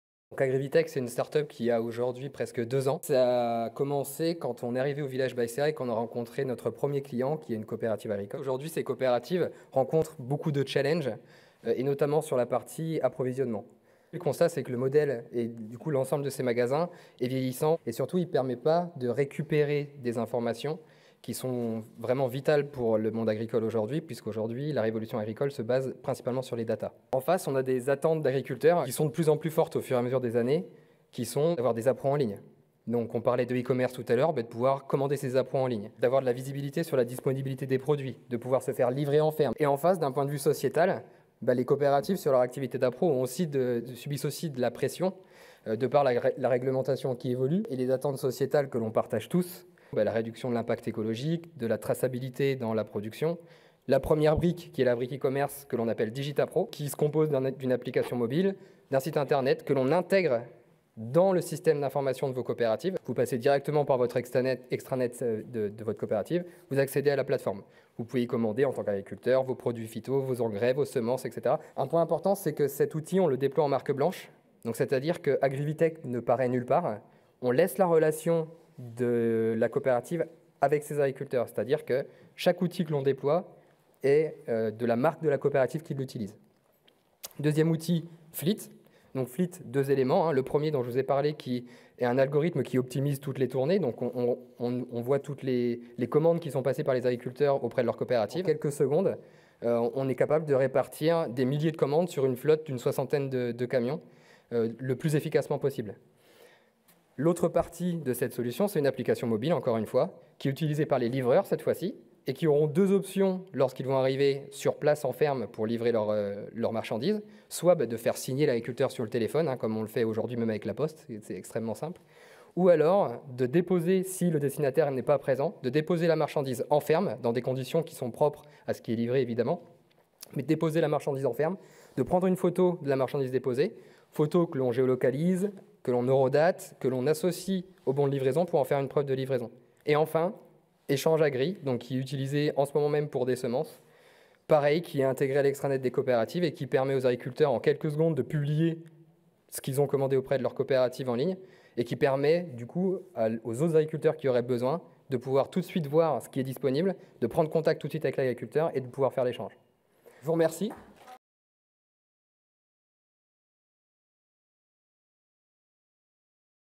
Intervention AgriVitech AG Crédit Agricole Avril 2022 (Version courte)
(Version courte)
Intervention de Nicolas Vilmart, PdG et CEO d''AgriVitech lors de l'assemblée générale du Crédit Agricole qui s'est déroulée au @villagebycareims6821  en avril.
Devant un auditoire attentif, cette prise de parole a été l'occasion de présenter l'éventail déjà fourni des solutions digitales dédiées aux coopératives et négoces agricoles, solutions créées et développées par AgriVitech.